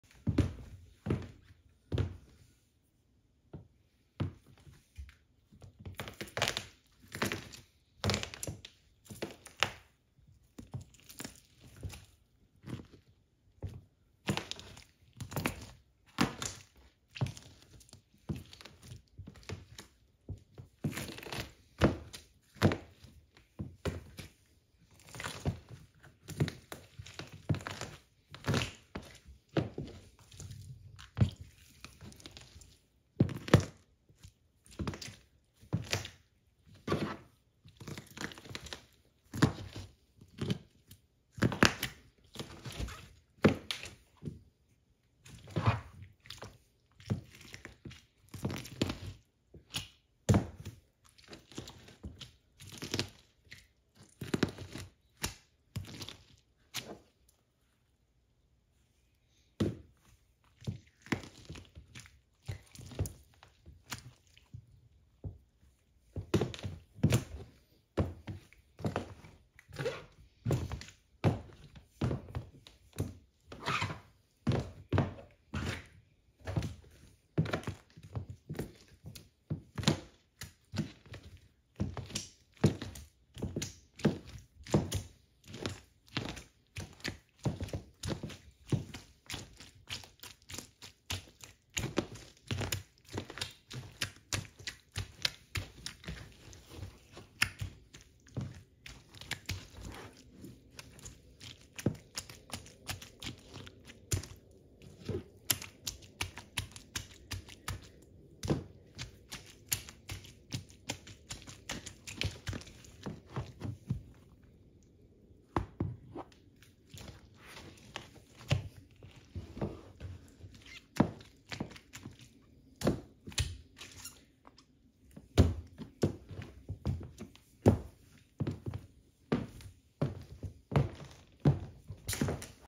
👠🍇 Grape CRUSH in Neon Yellow Heels | Strapy Sandals dominate fruit No Talk | Messy ASMR Squish
Please help grow the channel 😊